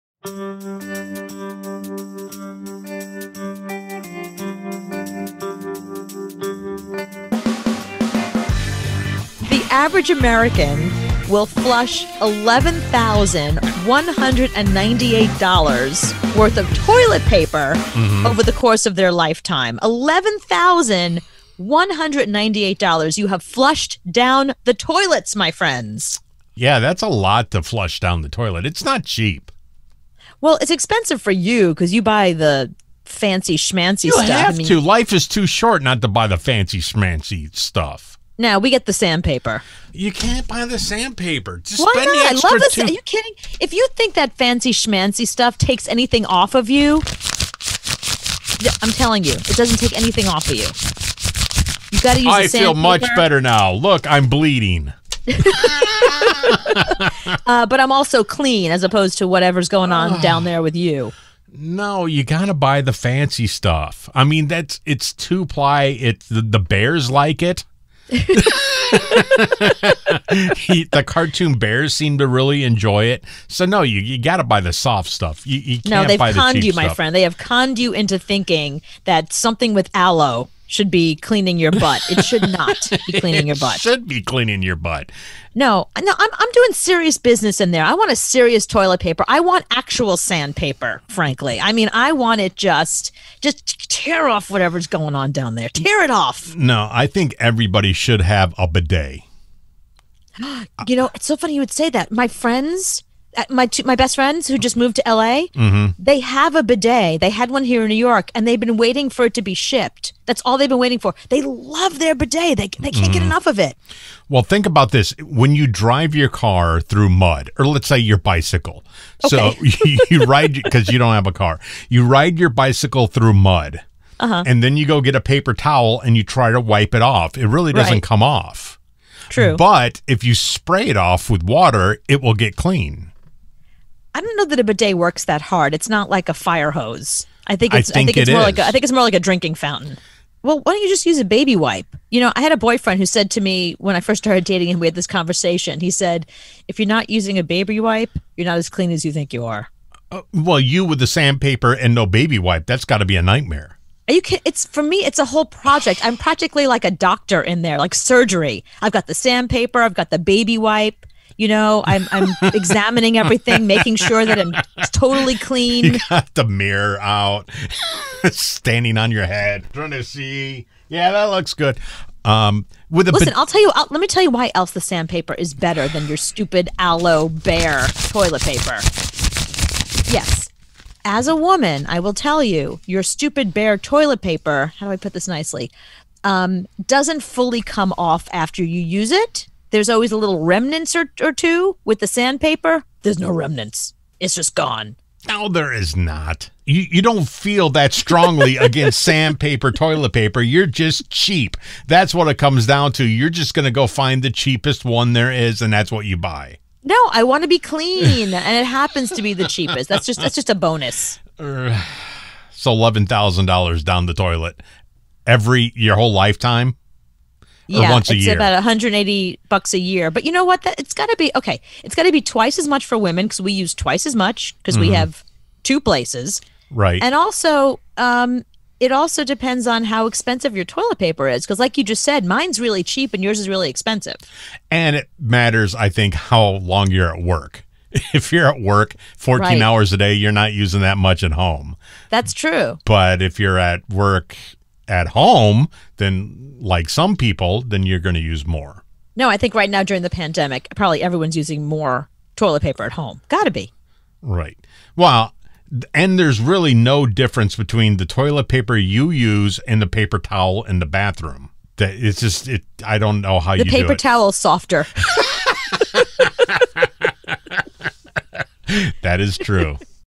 You'll Flush $11,198 Worth of Toilet Paper
The average American will flush $11,198 worth of toilet paper over the course of their lifetime, which works out to just over $180 a year.  But spending has almost doubled during the pandemic.